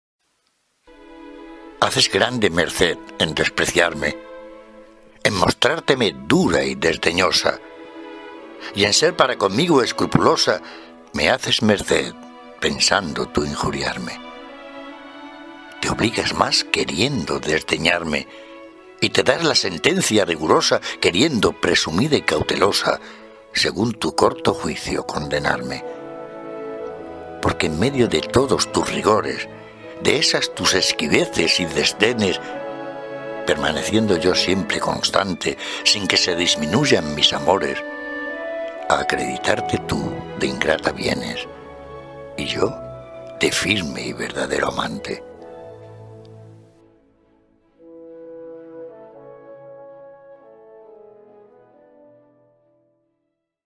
El poeta y su tiempo siglo XVIII prerromanticismo
Vídeo del poema "Haces grande merced en despreciarme" de Nicasio Álvarez de Cienfuegos, en la voz de José Luis Rico